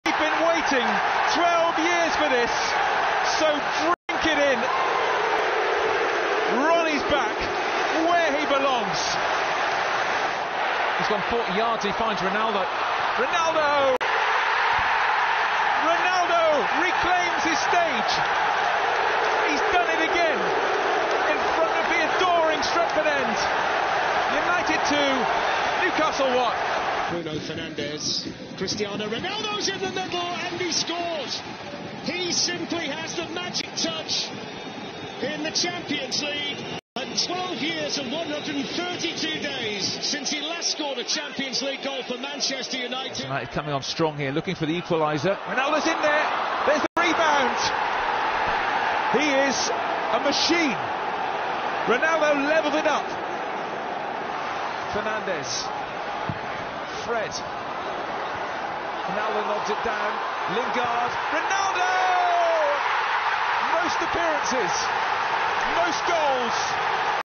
Cristiano Ronaldo's Incredible Champions League Comeback!
After 12 long years, Cristiano Ronaldo makes a triumphant return to the Champions League with a stunning performance for Manchester United. Witness his magic touch and goal-scoring prowess in this epic match against Newcastle.